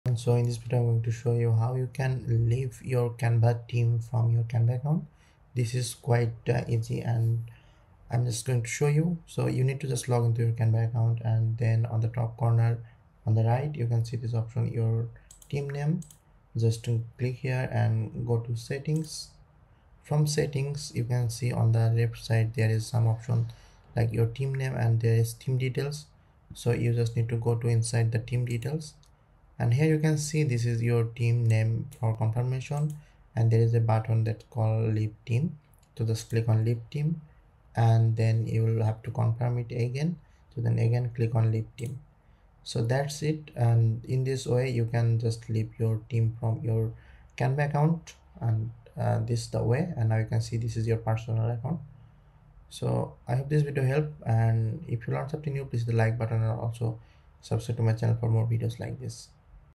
How to Leave a Canva Team in 2025
Learn how to leave a Canva team in 2025 with a step-by-step guide. Whether you're exiting a shared workspace or managing your account settings, this tutorial will show you exactly how to leave a Canva team in 2025 smoothly and hassle-free.

Key Points
Where to find the team settings in Canva
How to remove yourself from a Canva team
What happens after you leave a team
Important things to consider before leaving

Related Topic
How to Manage Canva Teams and Permissions